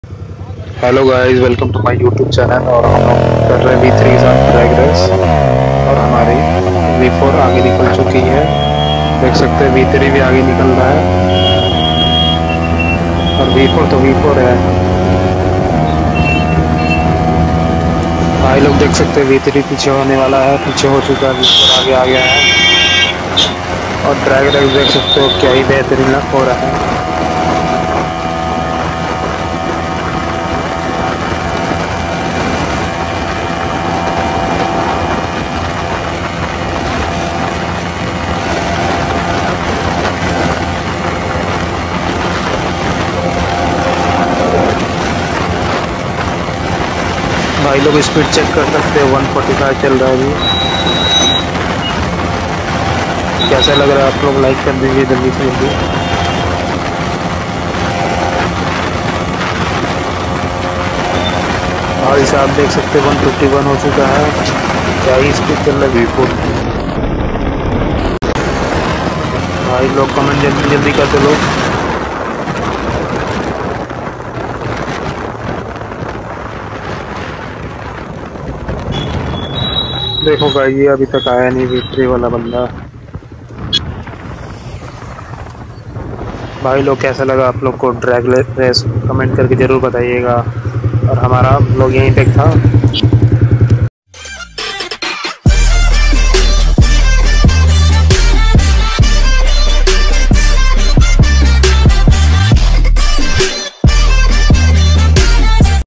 Yamaha R15 V4 vs Yamaha R15 V3 || Top End Race 🔥🔥 || must watch
The All New Yamaha R15v4 Vs Yamaha R15v3 Drag Race : Yamaha ने ग़लत किया R15 v4 vs r15v3 !!
R15 v4 vs r15v3